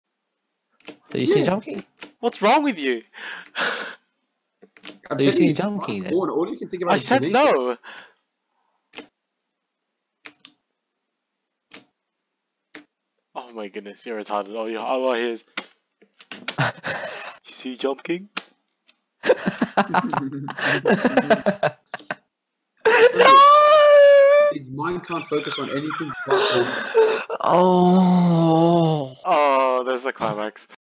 Joshua inquires if Sam has tried jump king
As a long day with the crew draws to an end, Joshua's mental capabilities receed exponentially.